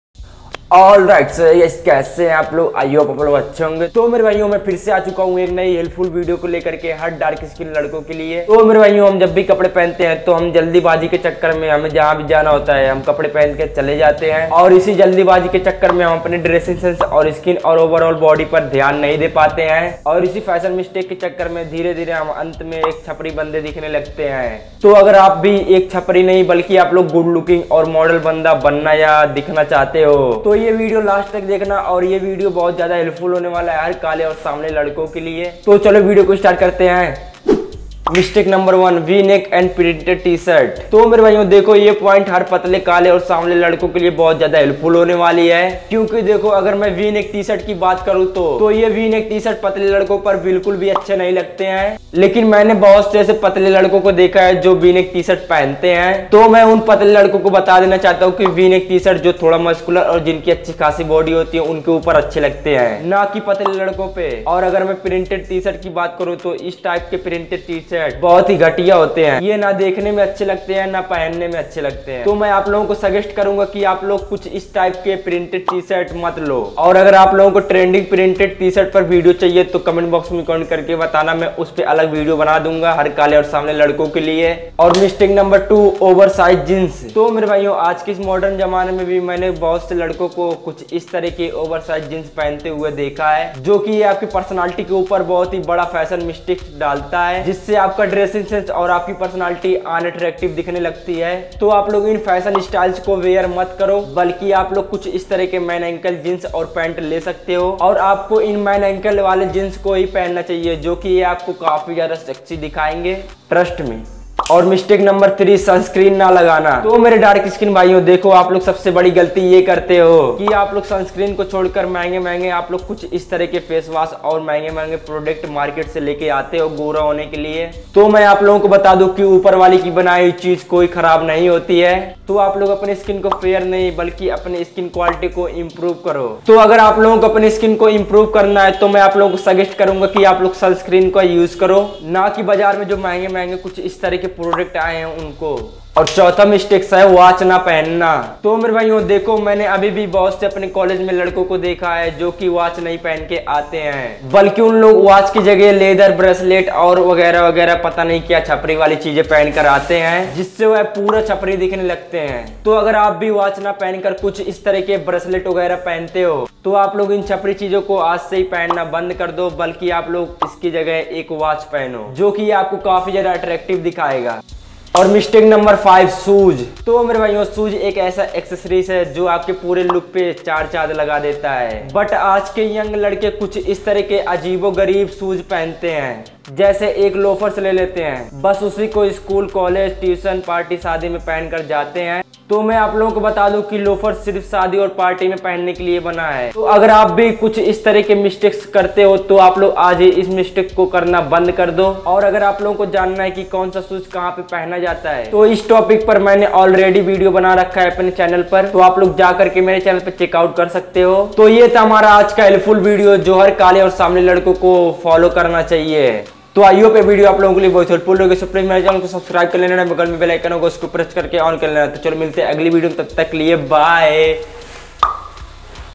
5 Fashion Mistakes Dark Men Should Never do | Dressing sense | In Hindi | Personality Development
About video👇

आज के इस वीडियो मे मैंने आपको बातया है की मेरे जितने काले या सावले भाई है वह किस Colour के और कौन से कपड़े को पहन कर कैसे आपने fashion को अपने पूरे personality को कैसे attractive बना सकते हैं और कैसे ठीक कर सकते है और आपने personality को develop कर सकते हो.